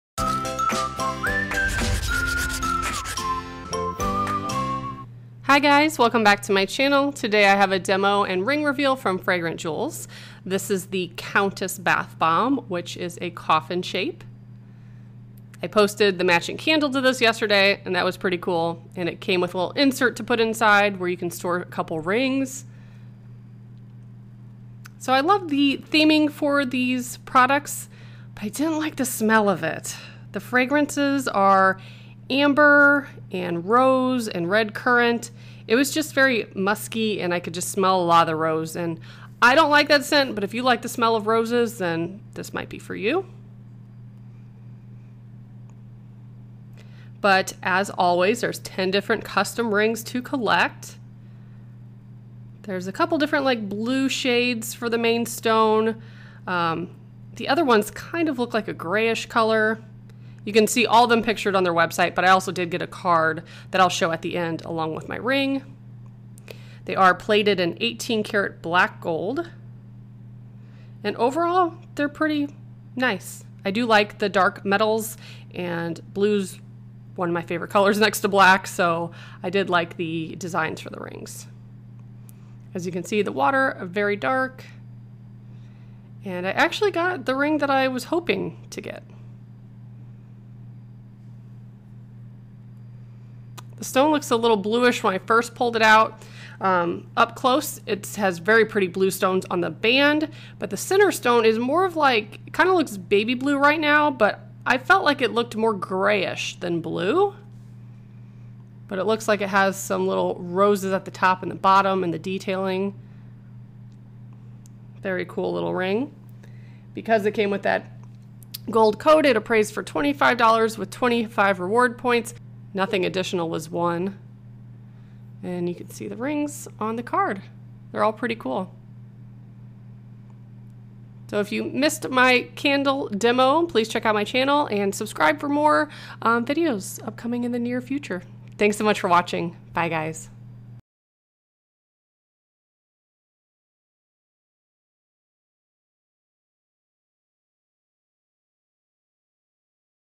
Fragrant Jewels - The Countess Bath Bomb DEMO and Ring Reveal!
Candle Warmer I use
My favorite bath pillow
Ebay Store

Today’s ring reveal came from the Countess Bath Bomb!

What’s on my nails? Jadey Business